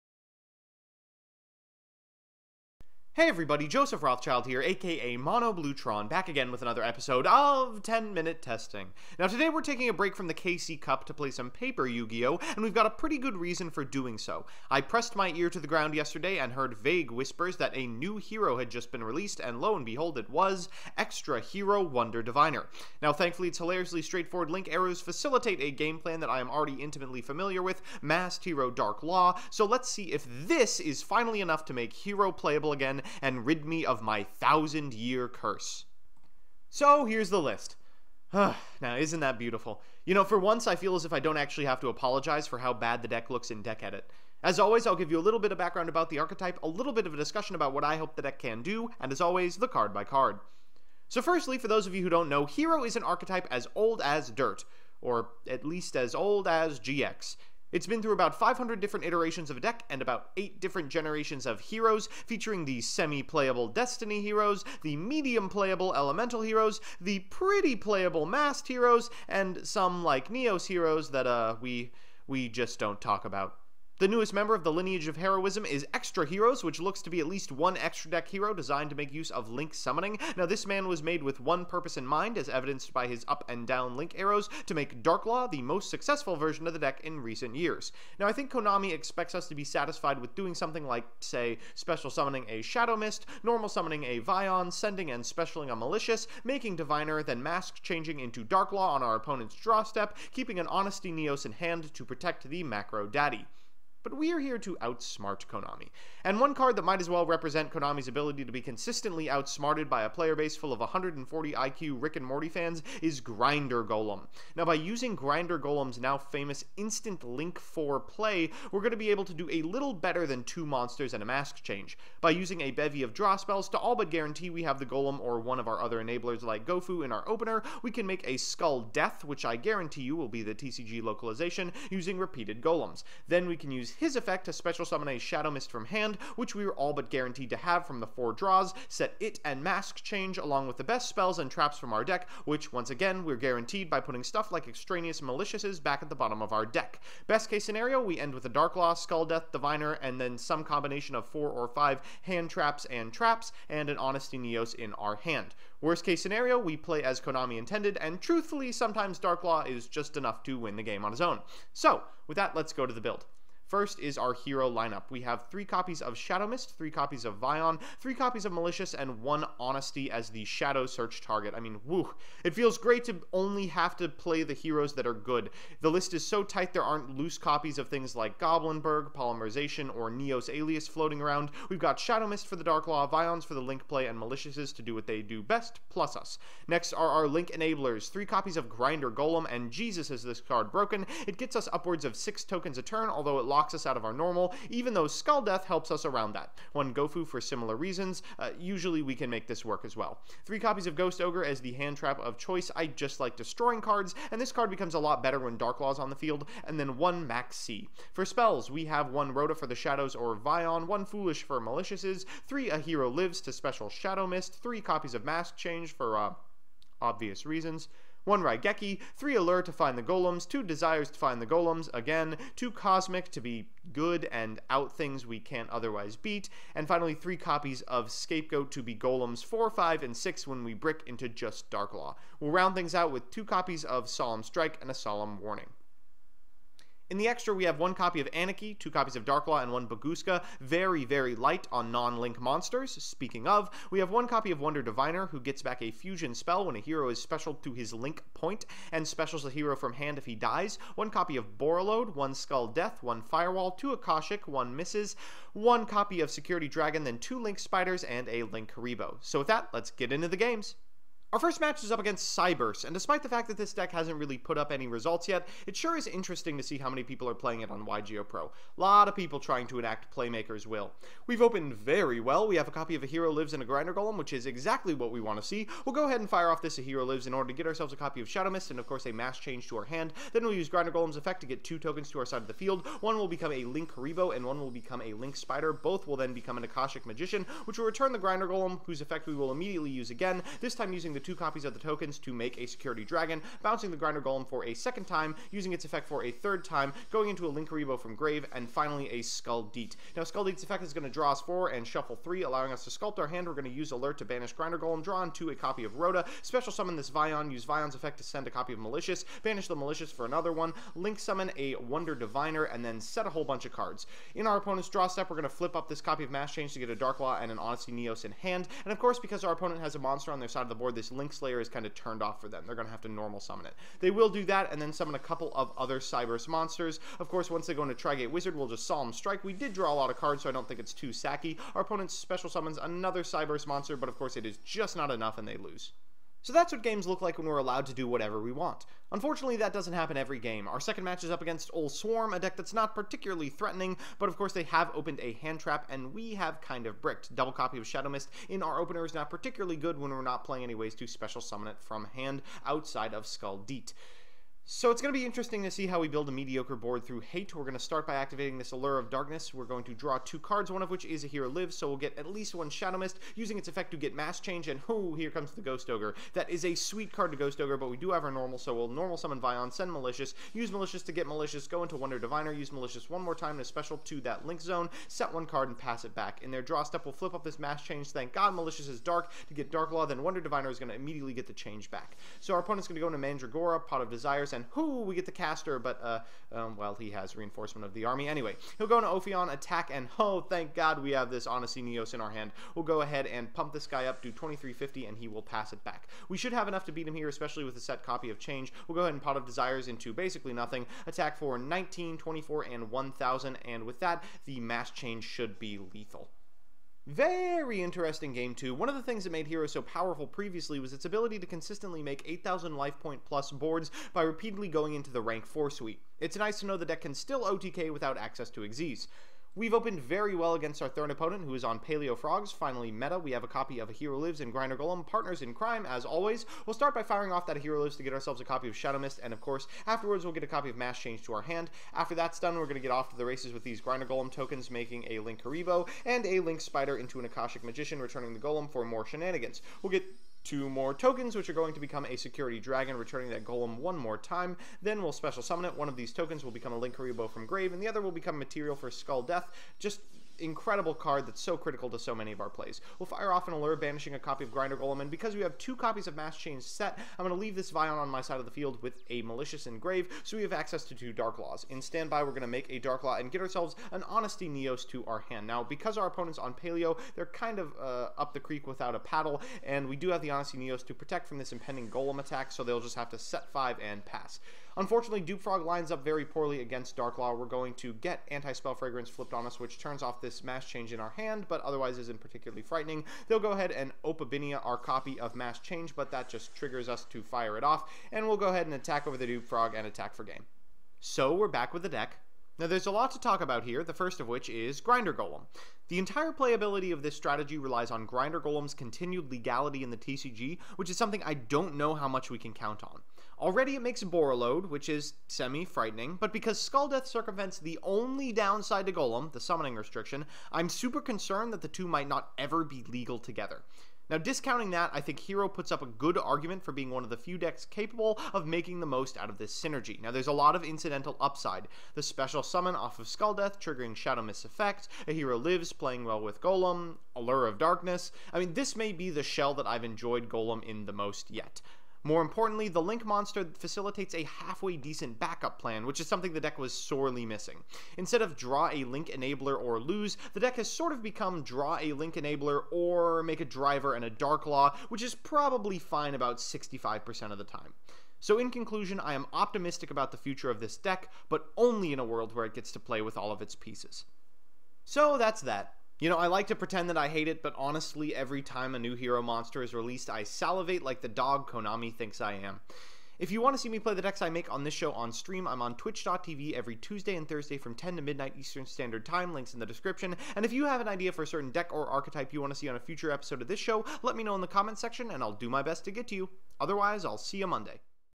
XTRA HERO - Ten Minute Testing 11/15/17
What's up?

"HERO..." I mutter, stumbling towards the run-down gas station for the twelfth time this month. This new HERO card might be just what the doctor ordered for one of the more beloved decks of all time, though if these duels are any indication, it won't stay beloved for long. Featuring Grinder Golem and Skulldeat, the Chained Dracoserpent, which really tells you all you need to know.